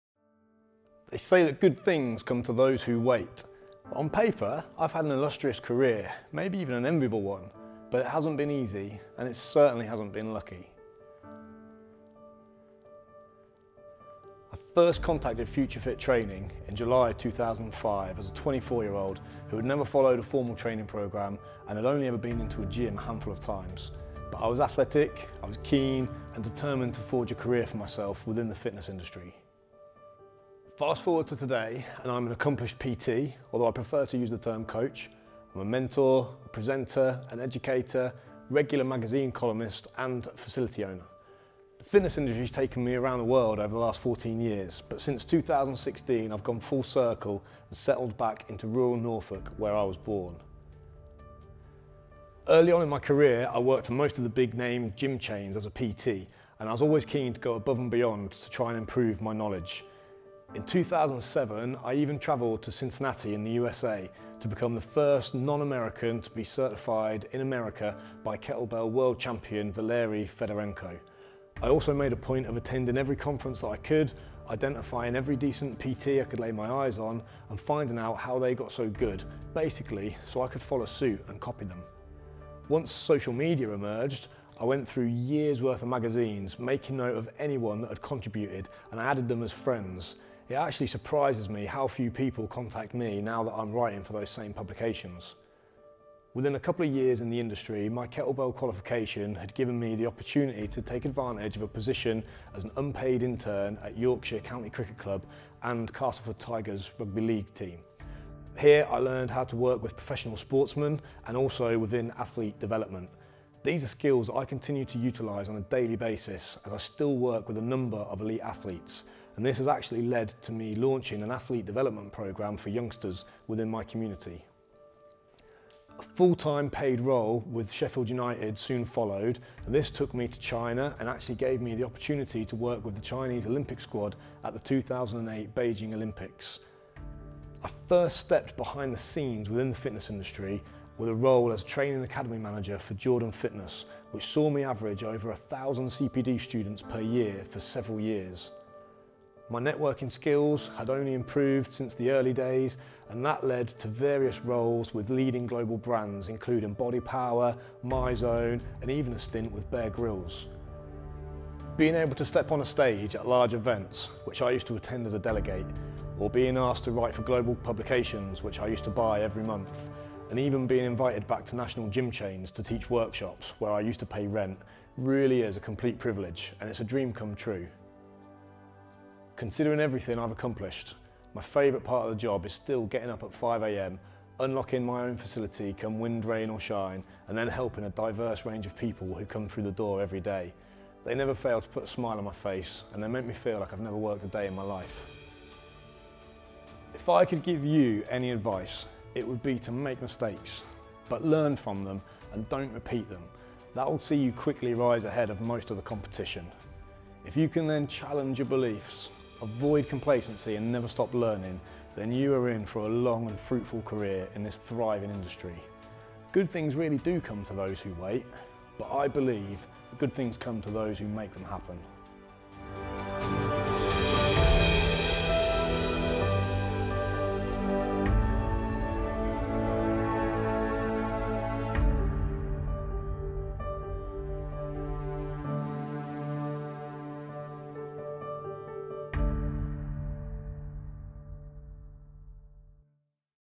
Mark Laws: My Story
"Considering everything I have accomplished, my favourite thing is to still to get up at 5am, unlocking my own facility come wind, rain or shine and then helping a diverse range of people. They never fail to put a smile on my face.” - Mark Laws

Future Fit Graduate explains how he turned his passion into a successful profession, having learnt from his mistakes. Mark is now a accomplished personal trainer, educator, columnist, presenter and facility owner! His hard work has led him to where he is today...